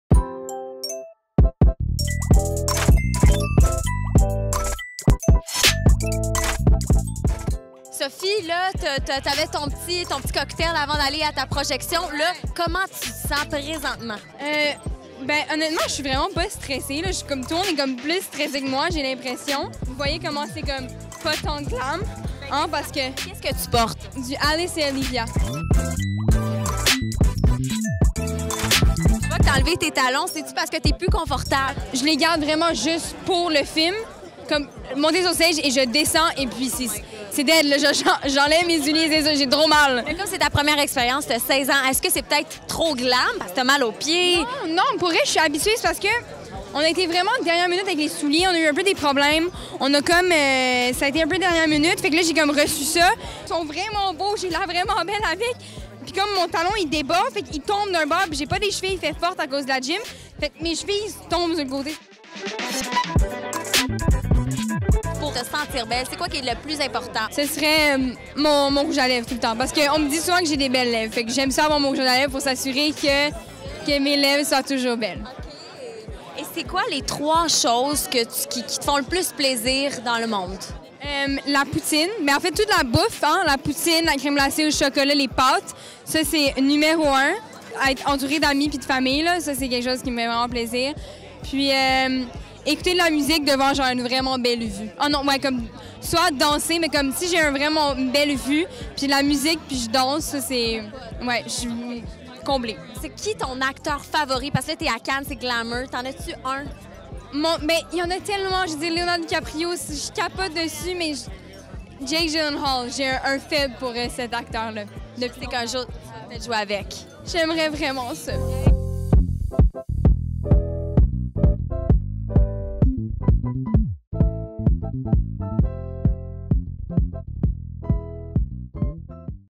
Cannes: nos collaboratrices de BangTV rencontrent l’actrice Sophie Nélisse
À l’occasion du Festival de Cannes, les filles de Bang TV ont rencontré Sophie Nélisse quelques minutes avant la projection du film Mean Dreams, dont elle est la vedette.